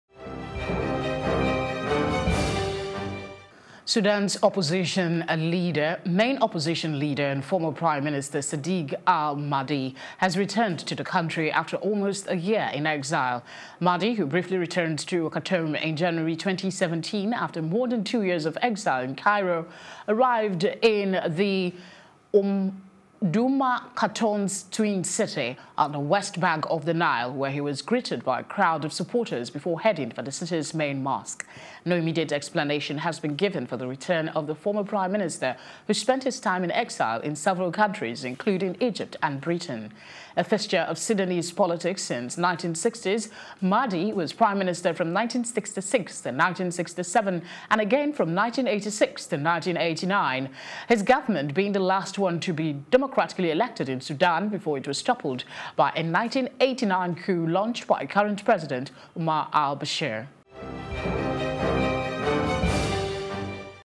Sudan: Opposition Leader Returns from Exile
Sudan's main opposition leader and former Prime Minister Sadiq al-Mahdi has returned to the country after almost a year in exile.